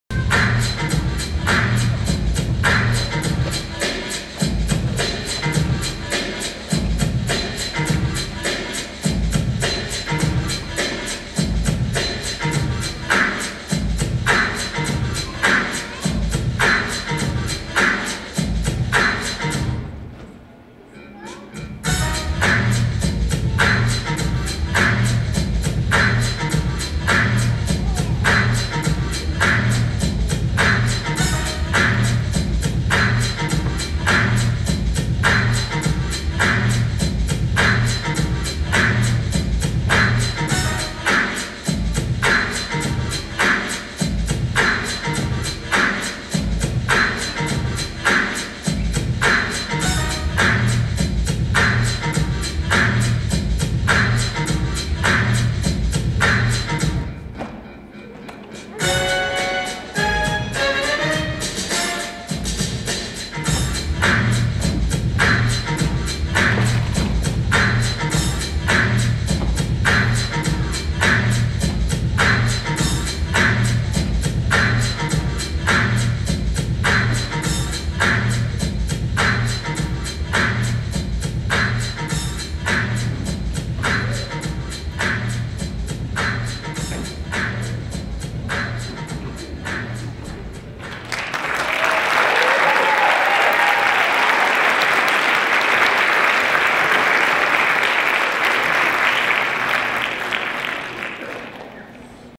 Fort Wayne Dance Collective - Christmas 2017 Performance Excerpt
This is an excerpt from the excellent 2017 Dance Collective Christmas Performance at South Side High School.  It was a great event for kids and adults.